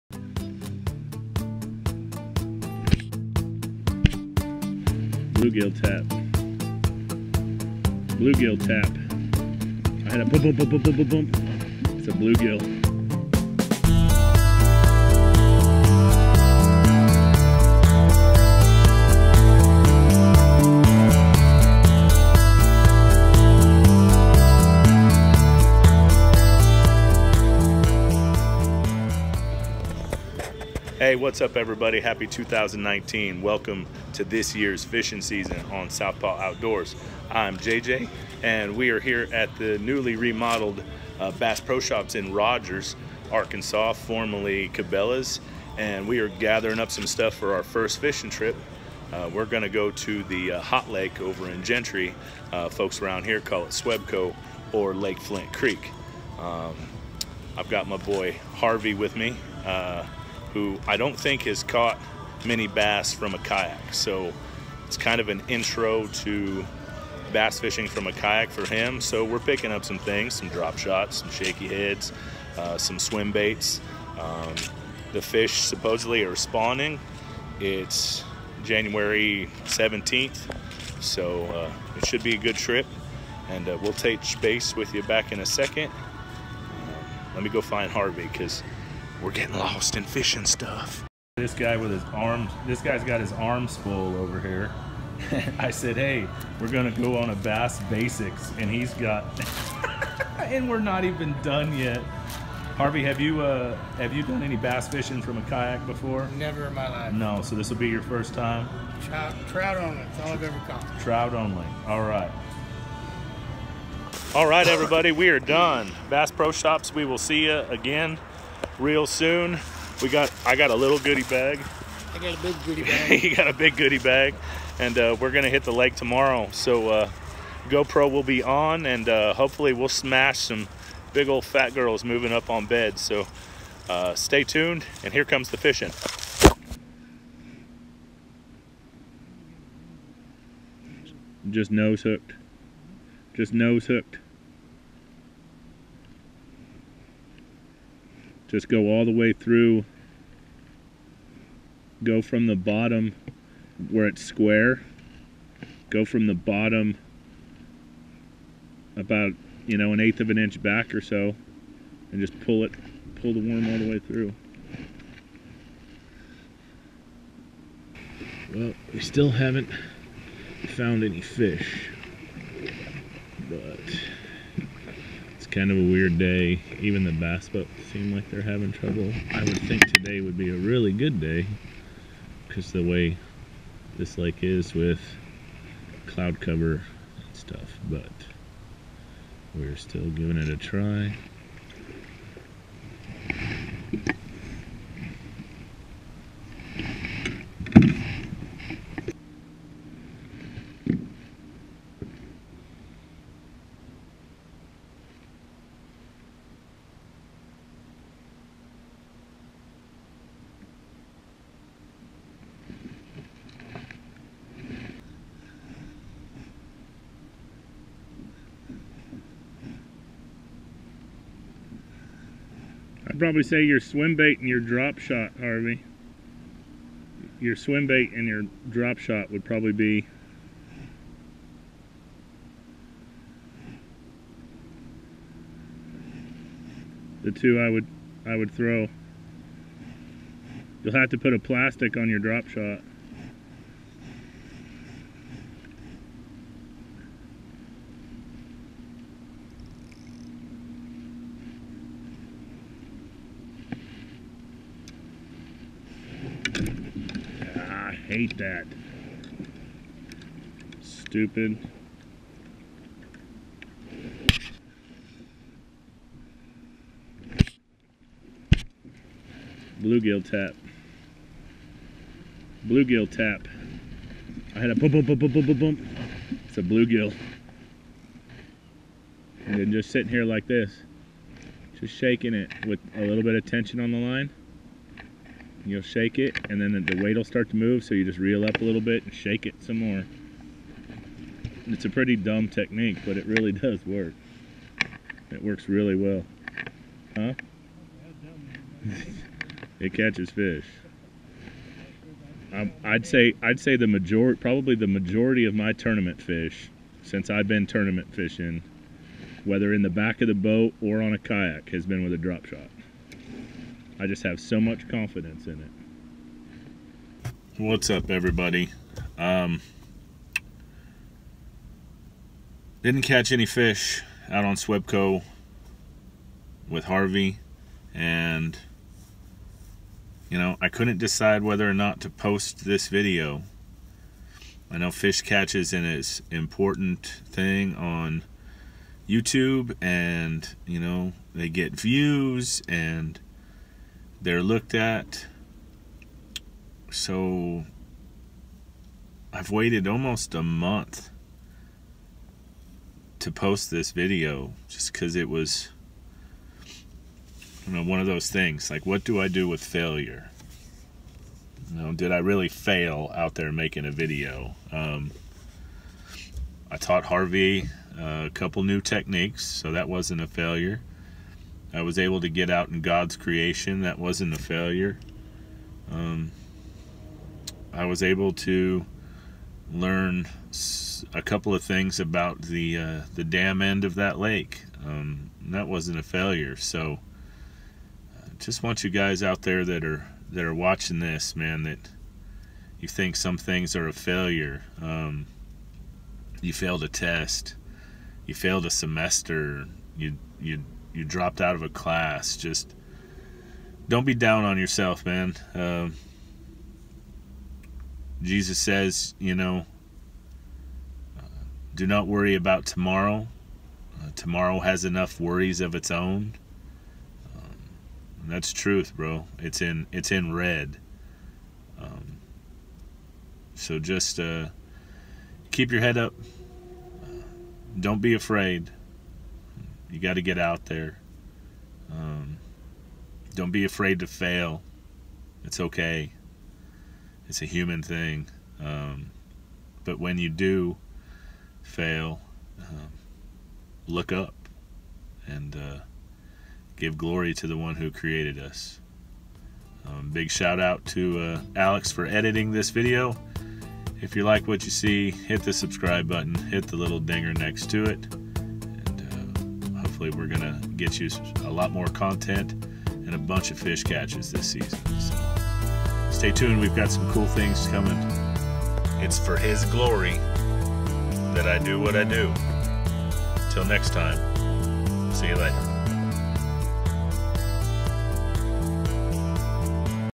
SWEPCO FAILURE?? OR WAS IT. JANUARY 2019
Bass fishing with brother Harvey on Swepco lake in Gentry 2019. It was a really tough morning. A lesson was learned though. God bless and be blessed.